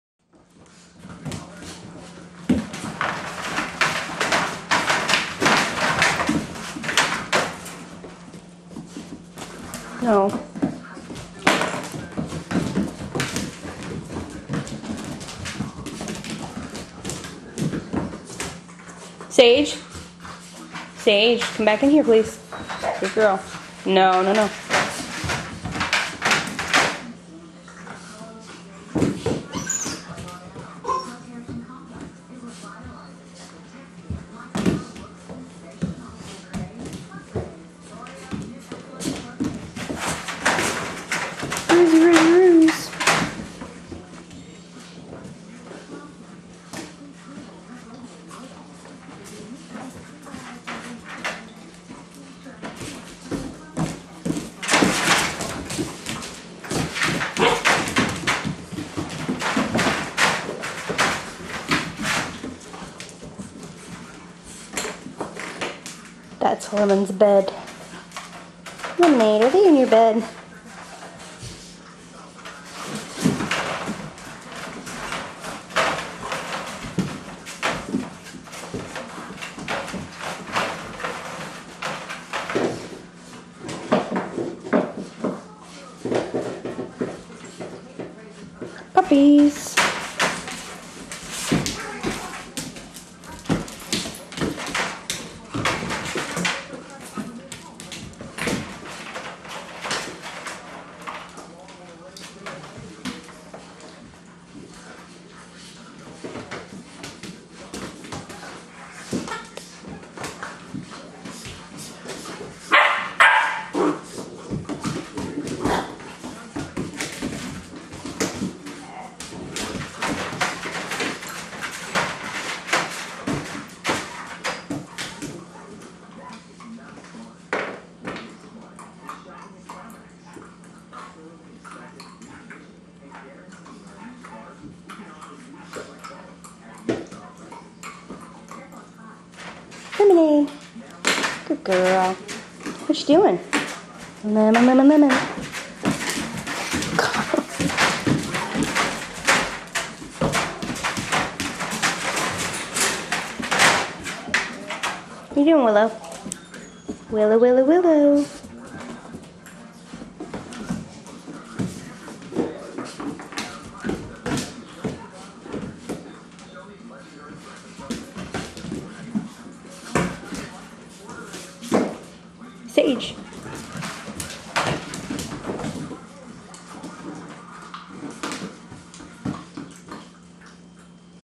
Crazy Puppies
My 11 week old GSP puppies have TONS of energy...this was shot after 25 minutes of outside run around like wild hellions time...